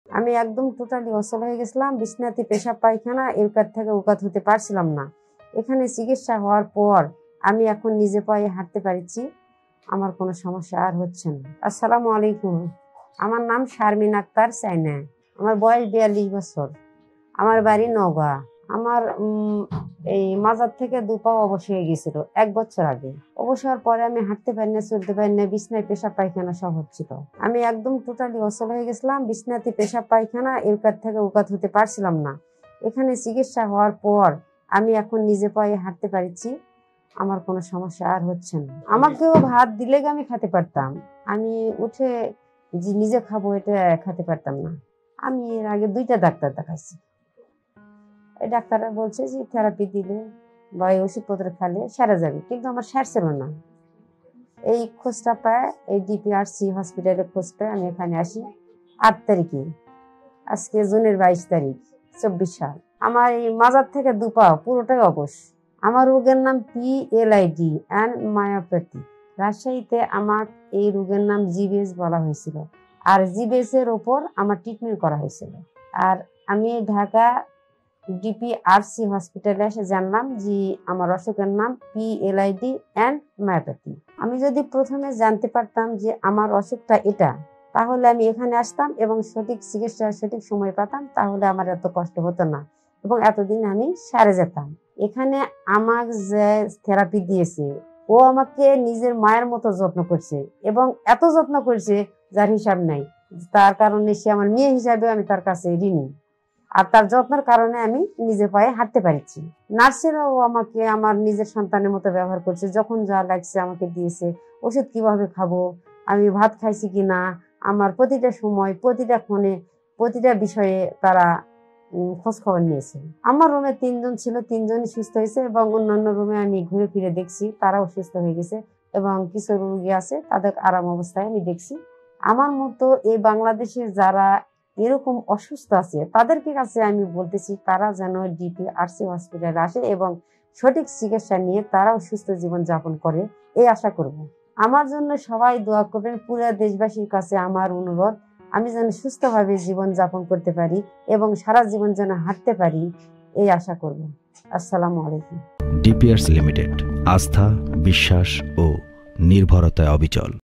মায়োপ্যাথিতে আক্রান্ত প্রায় পঙ্গু রোগী এখন সুস্থ | DPRC Hospital | Dr.Md.Shafiullah Prodhan | PLID
✺আমাদের  এর ঠিকানাঃ-
DPRC
12/1 Ring Road, shyamoli Dhaka-1207, Bangladesh.
(ডিপিআরসি , 12/1  রিং রোড শ্যামলি ঢাকা-১২০৭ বাংলাদেশ)